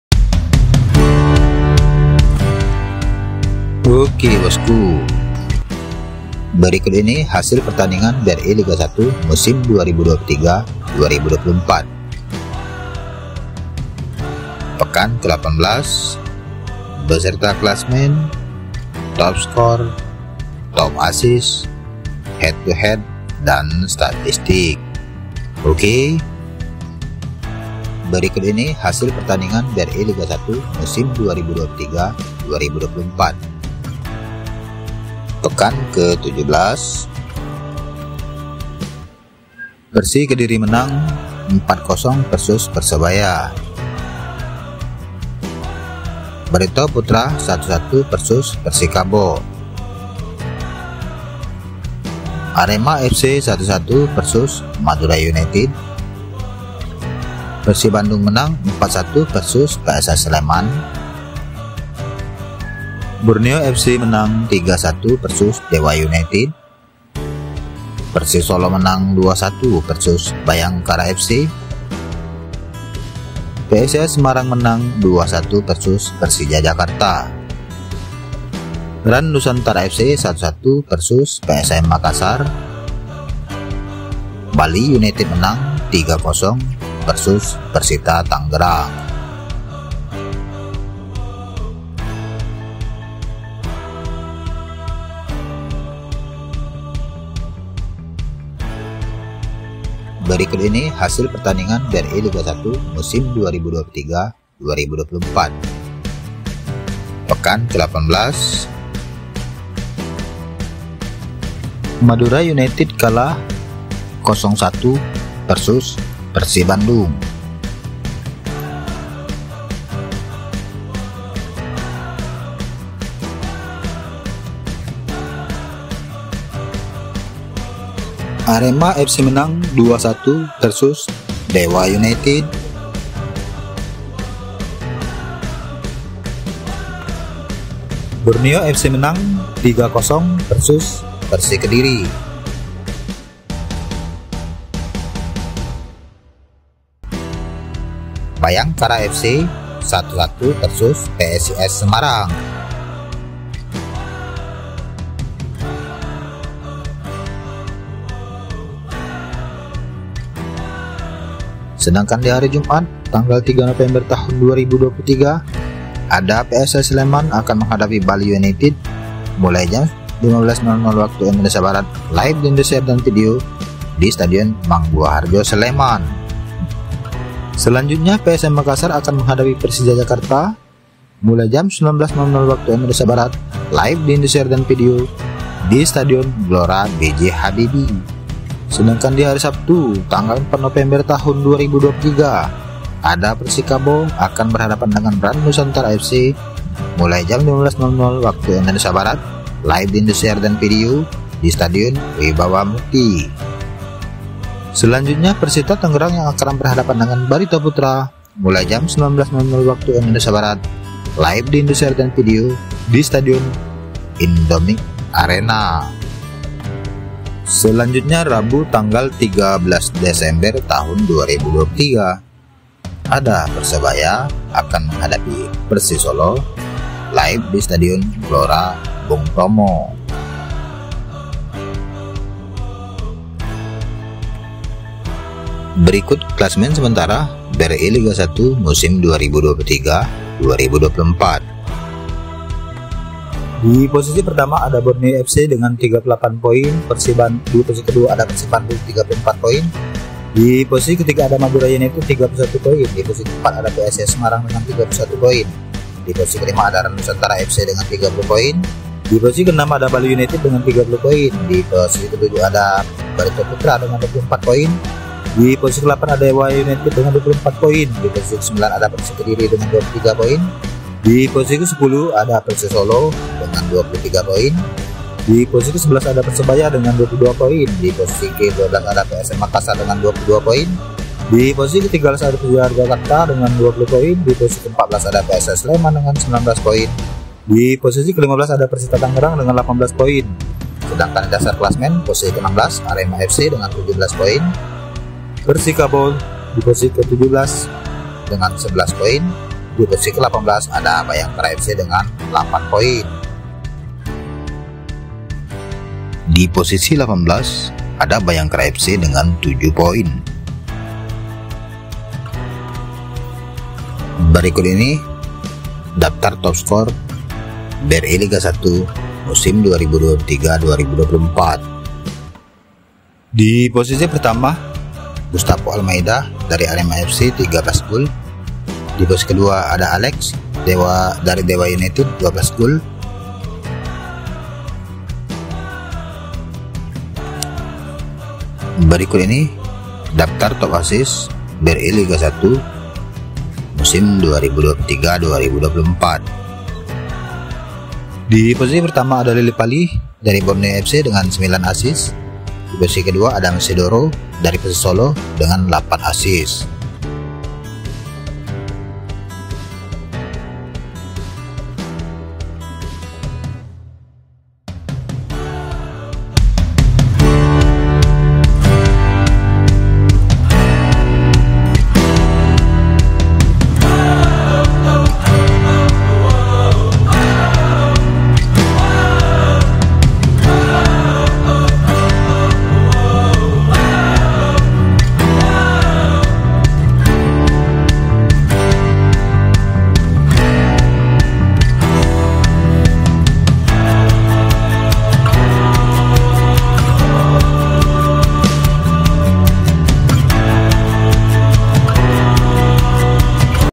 Hasil Liga 1 2023 Pekan ke 18 - Borneo vs Persik- Bhayangkara vs PSIS - BRI Liga 1 2023
Berikut hasil liga 1 hari ini kamis 2 november 2023:
AREMA FC 2-1 DEWA UNITED
MADURA UNITED 0-1 PERSIB
BORNEO 3-0 PERSIKEDIRI
BHAYANGKARA 1-1 VS PSIS SEMARANG

SEDANGKAN DI HARI JUMAT 3 NOVEMBER  TAHUN 2023
PSS SLEMAN VS BALI UNITED
PSM MAKASAR VS PERSIJA JAKARTA

DAN PADA HARI BERIKUTNYA SABTU 4 NOVEMBER
PERSIKABO   VS RANS NUSANTARA FC
PERSITA VS BARITO PUTERA

PEKAN KE 18
RABU TANGGAL  13 NOVEMBER  TAHUN 2023
PERSEBAYA  VS PERSIS SOLO